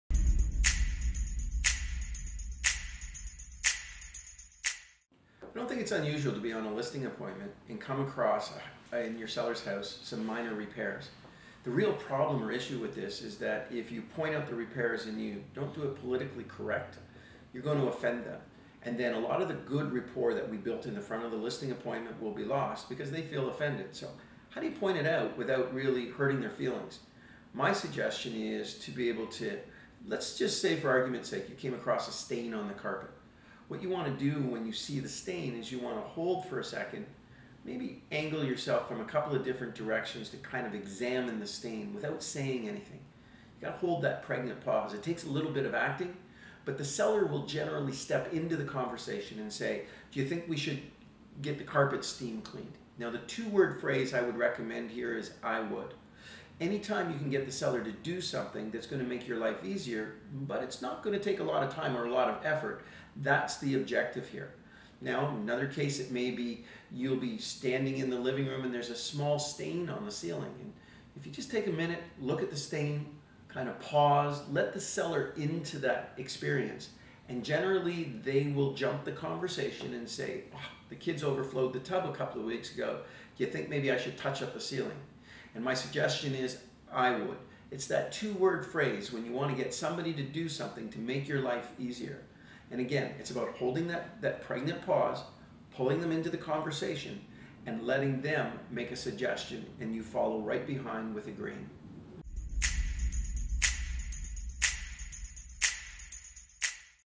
Realtor Advice - How to tactfully point out repairs
It’s not unusual to be on a listing appointment and come across some minor repairs in your seller’s home. You don’t want to offend them by drawing attention to the issues because it could potentially ruin the solid rapport you’ve already built, but it would still help with the sale if they addressed some of the problems. For TalkShopTuesday, real estate trainer and coach Chris Leader talks about how he would handle it.

For more exceptional real estate advice, check out Leader's Edge Coaching